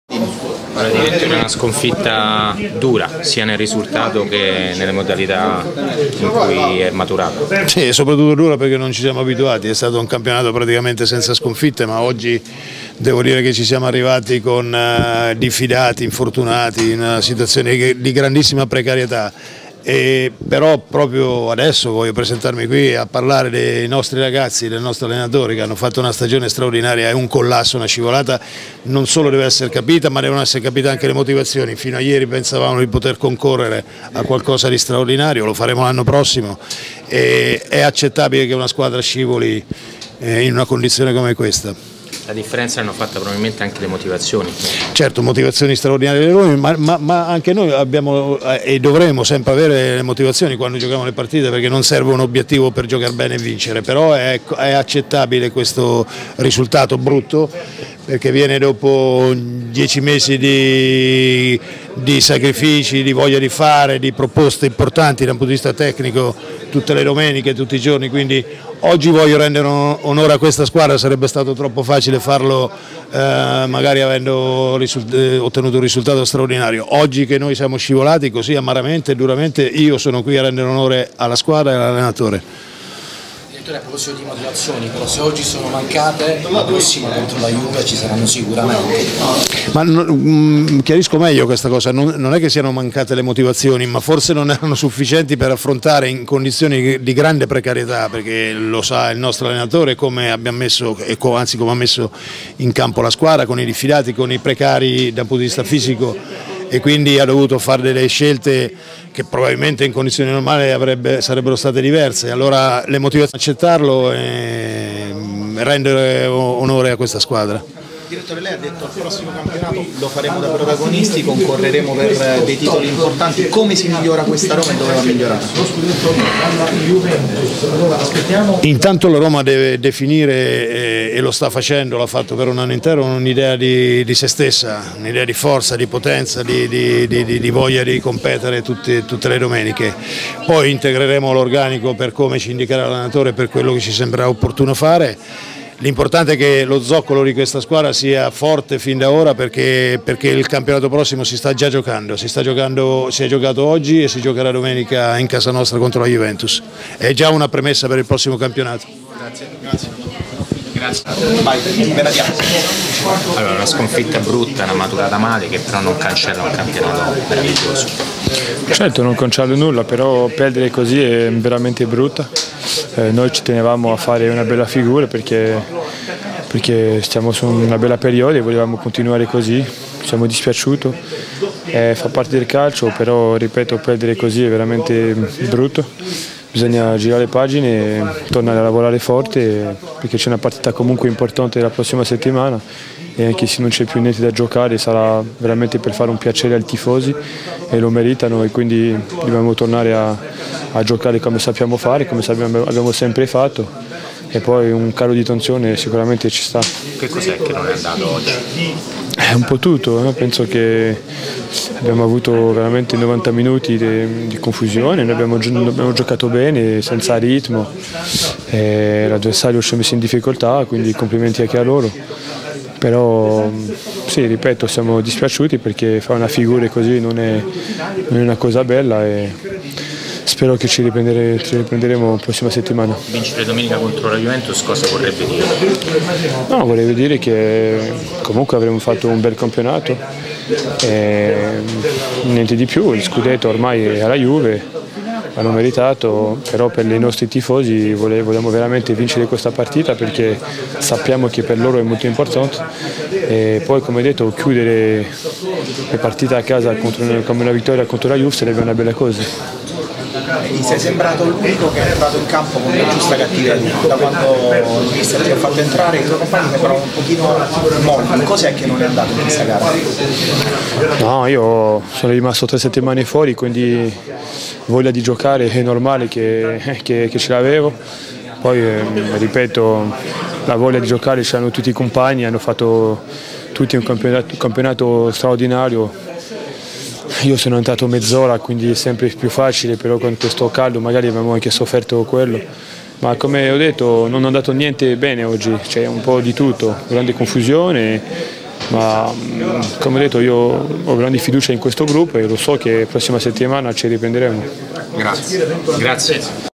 Catania - Roma 4-1 / Sabatini e Benatia in mixed zone
Le interviste nel dopo gara di Catania - Roma effettuate dal nostro Federico Lo Giudice